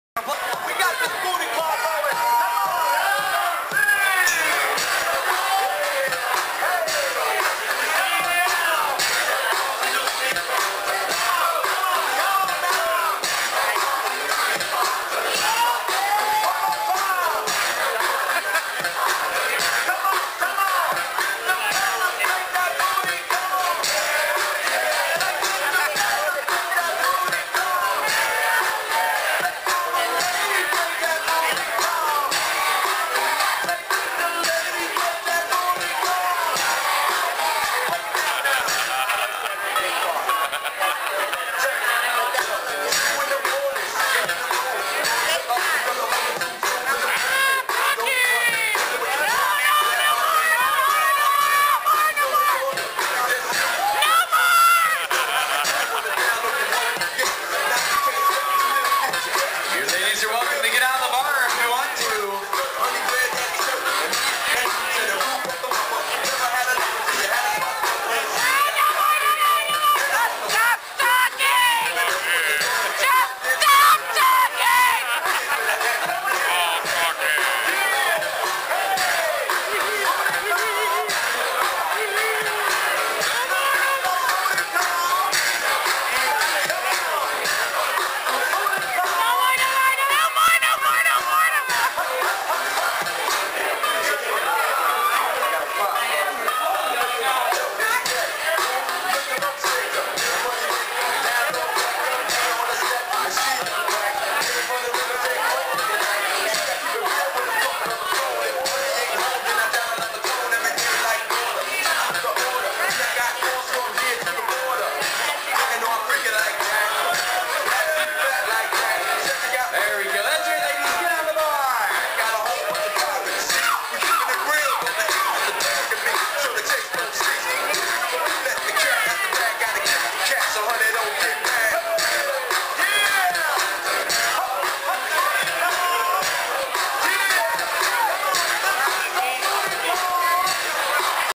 Goofing Sc Shores Inn 030710.wmv
Karaoke Night Shores Inn 030710. Never give Scott the Camera! LOL!